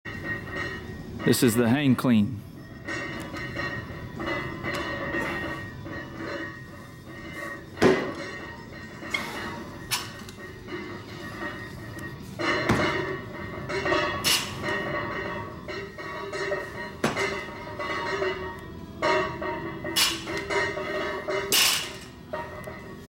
HANG CLEAN
Grip bar with a clean grip and lift bar from the floor with a clean pull.  Stand erect, then bend the knees and let the bar dip about 5 inches with the shoulders over the bar.  At this position, jump and shrug the shoulders hard.  Keep the weight close to the body during the climb of the bar and the elbows over the wrists during the pull. Rack the weight on the shoulders, and then repeat the movement.  Feet should be shoulder width apart. Hand spacing should be directly outside the thighs.  This is a quick explosive movement. Variation - Hang Clean and Jerk.